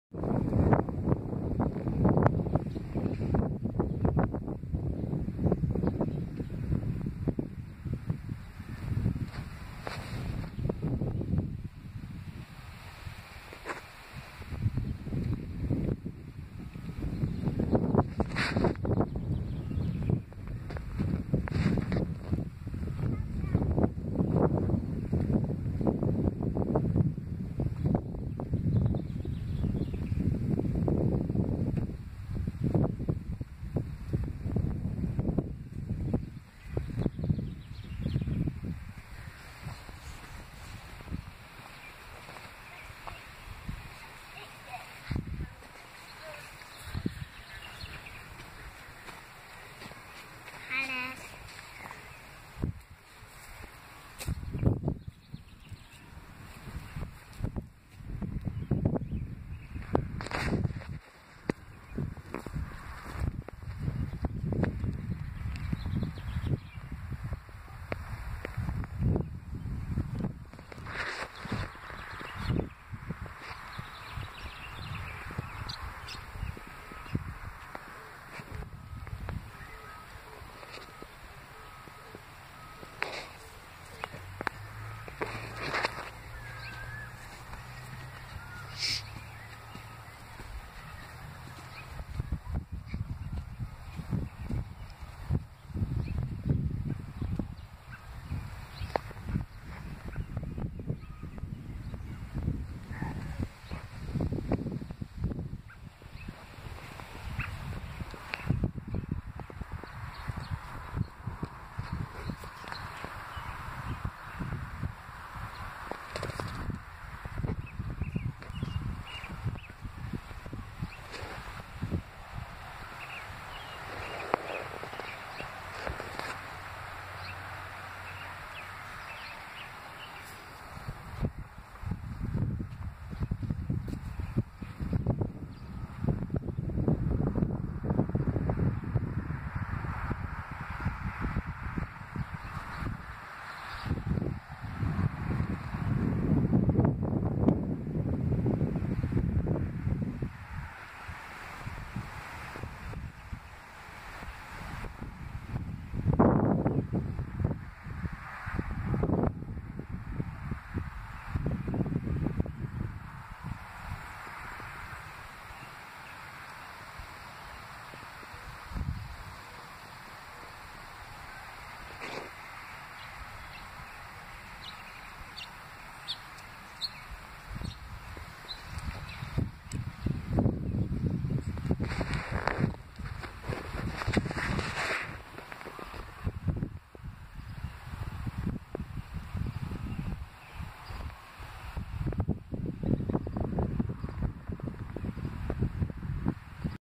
Blossoms everywhere
The fruit trees are really blossoming.  Sometimes when the wind blows it looks like it is snowing because of all the blossoms blowing around.